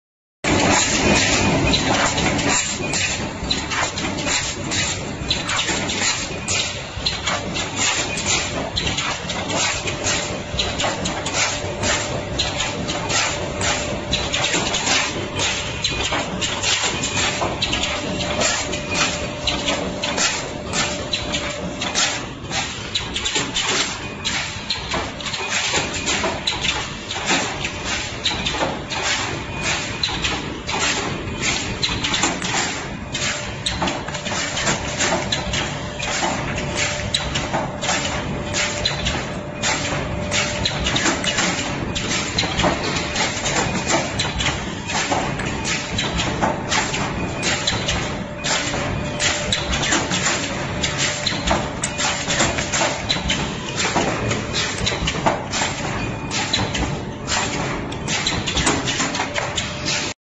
New technology Hydraulic Paper plate tray dish forming machine Feenot
Paper plate tray dish forming machine Feenot

Making paper plate dish tray bowl container,

Speed: 60~120 pcs/min (two station)

FeeNoT.com